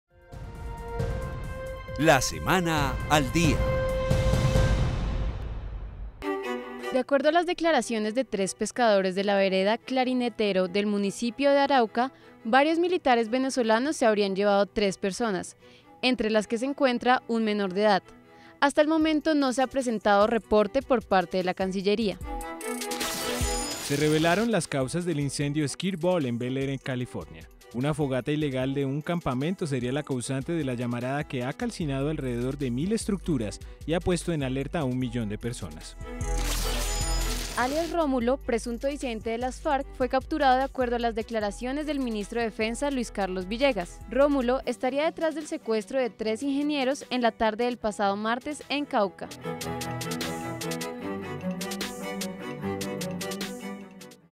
Una fogata ilegal fue causante del incendio que calcinó más de mil estructuras
La actualidad noticiosa de Colombia y el mundo está aquí, no se pierda Pongámonos Al Día